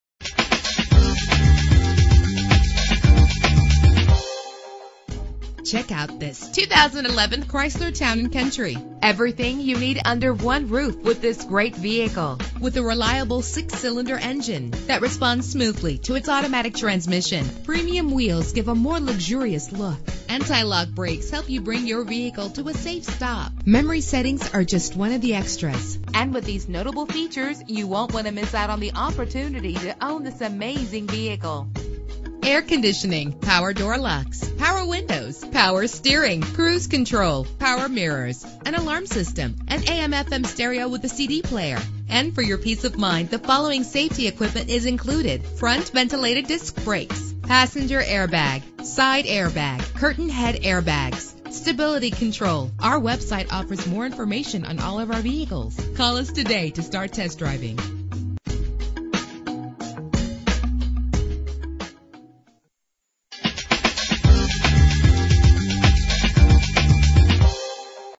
2011 Chrysler Town & Country - St. Charles IL
Year: 2011
Make: Chrysler
Model: Town & Country
Trim: Touring
Engine: 3.6 liter 6 cylinder 24 valve
Transmission: Automatic
Color: Deep Cherry Red Crystal
Mileage:
Address:
1611 East Main St.